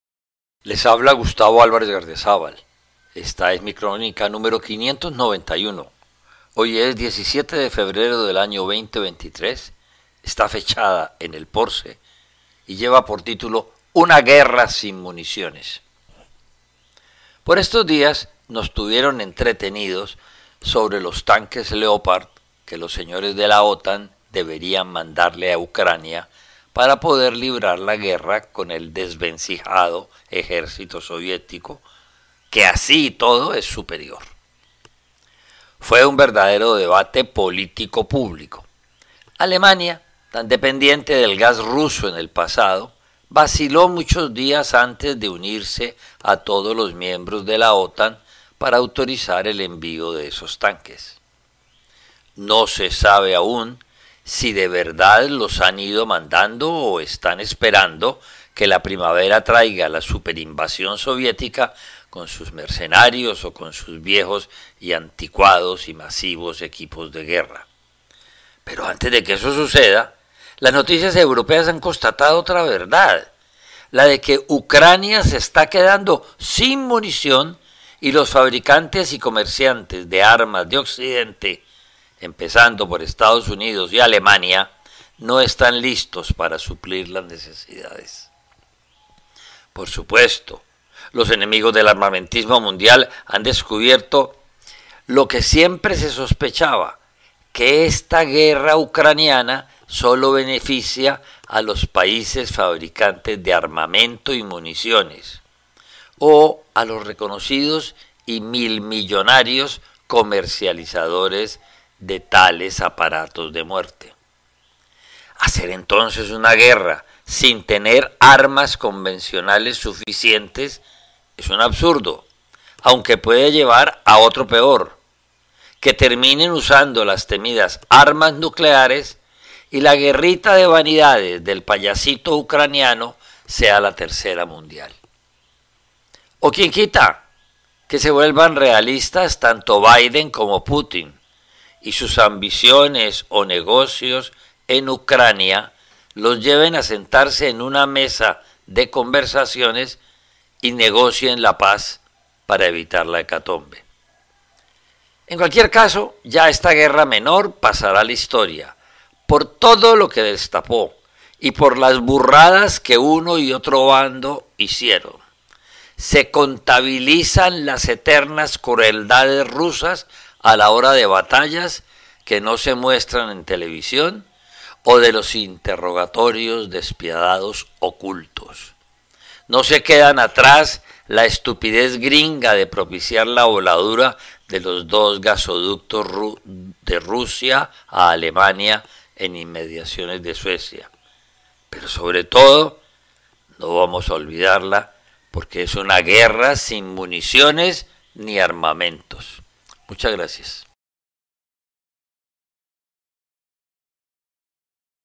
Una guerra sin municiones
El maestro Gustavo Álvarez Gardeazábal analiza a la guerra de Ucrania, que se está quedando sin municiones.